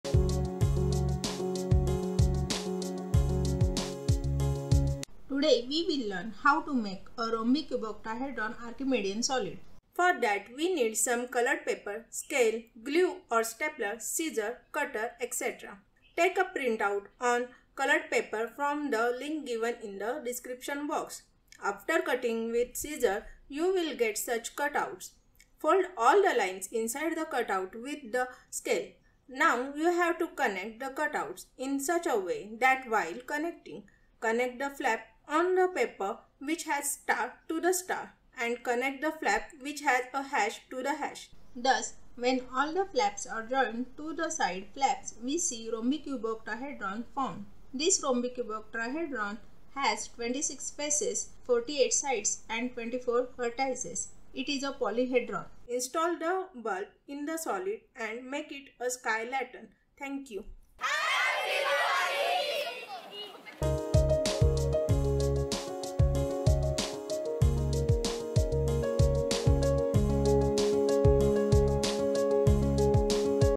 Archimedean Lantern | Rhombicuboctahedron | English
Today we will learn how to make a Rhombicuboctahedron. For that, we need some coloured paper, a scale, glue or a stapler, scissor etc. Take a printout on coloured paper from the link given in the description box. After cutting with scissors, you will get such cutouts. Fold all the lines inside the cut-out with the scale. Now you have to connect the cutouts in such a way that while connecting, connect the flap on the paper which has a star to the star and connect the flap which has a hash to the hash. Thus, when all the flaps are joined to the side flaps, we see Rhombicuboctahedron formed.
This Rhombicuboctahedron has 26 faces, 48 sides and 24 vertices. It is Polyhedron.

Install the bulb in the solid and make it a sky lantern.

Team: Prof. M.S. Santhanam, Prof. Kundan Sengupta, Dr. Aparna Deshpande, Shubhangi Wankhede, Ashok Rupner, Dr. Chaitanya Mungi, Ankish Tirpude, Tejal Vyas, Neha Apte, Rashmi Bahulkar, Manisha Ghale and Rohit Piwal.